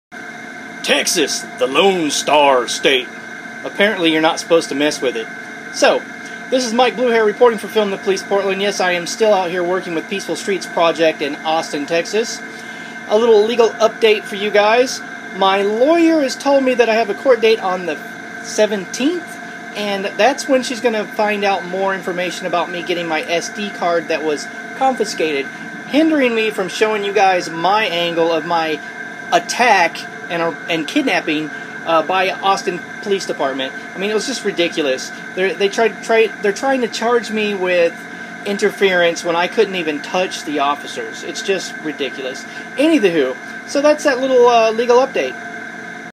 Another legal update from my Texas Arrest
I believe the charges filed against me by the Austin Police Department are unfounded. I also believe that the multiple videos taken that night from other people corroborate my side of the story. I also believe I was targeted for arrest because I was filming the cops. Dirty unethical law enforcement personnel try everything in their power to avoid public scrutiny. Bad apples don't like being scrutinized or held accountable for their actions.I was knocked down by Austin PD for nothing more than reaching my hand out. My intentions were to make sure Antonio's camera did not get smashed or hit the ground.At no point was I close enough to the officer To actually grab my friend during his arrest. if I were actually physically resisting then they would have grounds and probable cause to arrest me for interfering. Donating to my cause shows that you're taking a stand with me against injustice. when I stand up and flex my right I preserve and defend the rights of everyone. Democracy is a contact sport! sometimes people like myself have to push the envelope and get into the court system and let a jury of my peers determine my fate. In doing so I preserve and defend the rights of all of us. If I am unable to afford my attorneys fees I'll never get my chance to have my day in court. personally I am very proud to stand up for the first amendment of the U.S. Constitution. The Supreme Court has ruled numerous times that documenting police activity in public is a first amendment right. we will all lose this right if people don't exercise it. I am willing to put my body as well as my freedom on the line to preserve this right. Who will stand with me? consider looking over my legal defense page! thanx
-Mike BlueHair